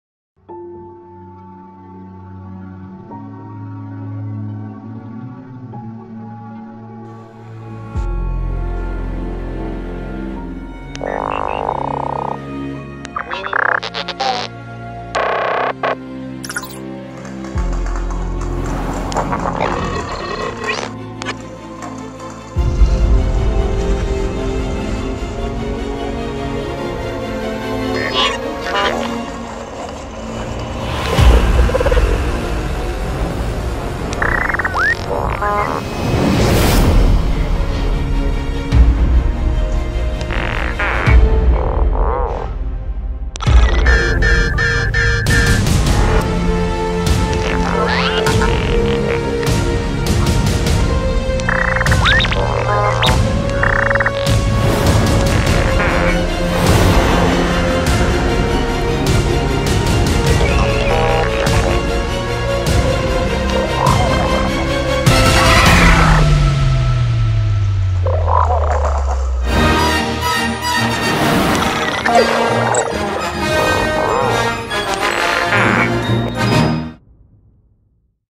BB 9E App Enabled Droid
There’s a new disturbance in the Force. BB-9E™ is a menacing Astromech Droid of the First Order. Control your BB-9E App-Enabled Droid with your smart device or watch it patrol on its own. BB-9E houses sophisticated tech, allowing it to roll and move his dome just like on-screen, and its strong exterior allows the First Order Droid to weather any battle. This vigilant and intimidating Droid is always on high alert.

BB-9E specializes in keeping starships and machinery fully operational with its many features. Keep BB-9E sharp with the Droid Trainer and explore holographic simulations from the Star Wars™ galaxy. Watch BB-9E interact with other Star Wars App-Enabled Droids by Sphero, and view films from the Star Wars saga with BB-9E reacting by your side. This is NOT the Droid you’re looking for… it’s the Droid that’s looking for you.